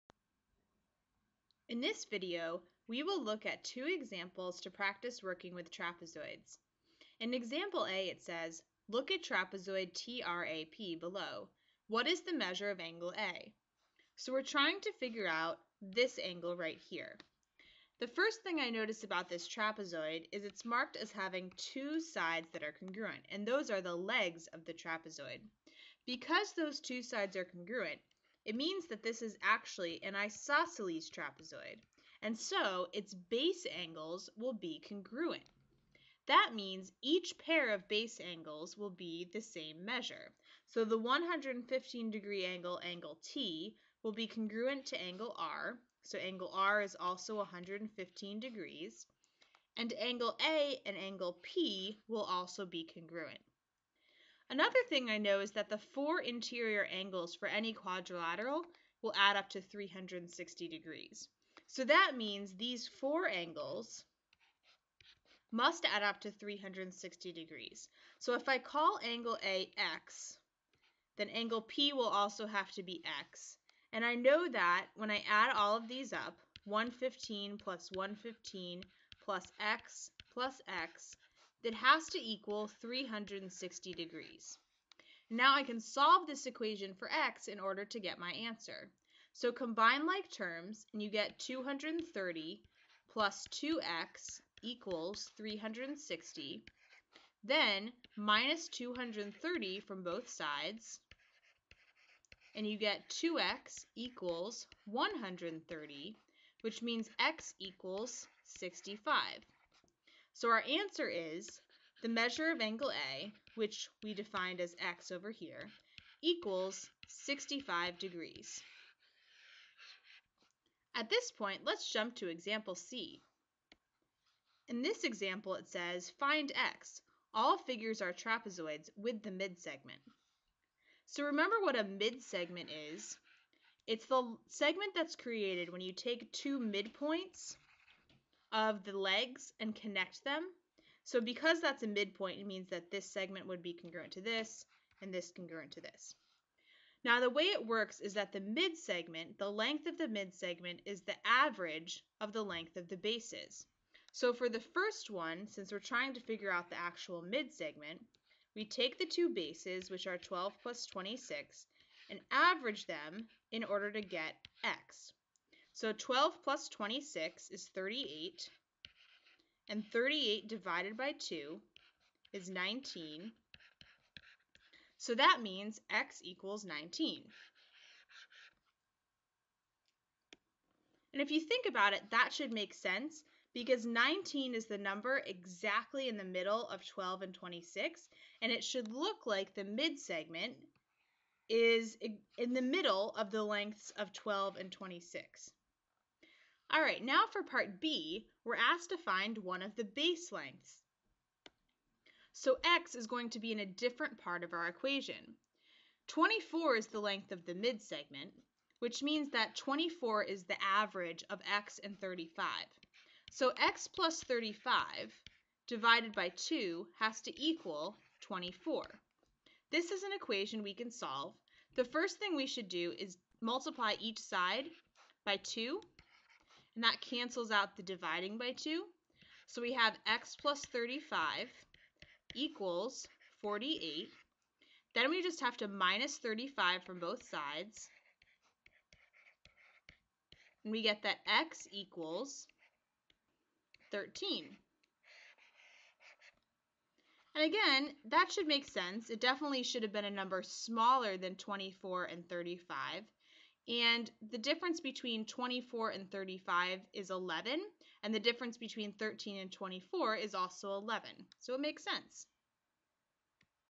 Trapezoids: Examples (Geometry Concepts)
Here you'll learn the properties of trapezoids and how to apply them.

This video shows how to work step-by-step through one or more of the examples in Trapezoids.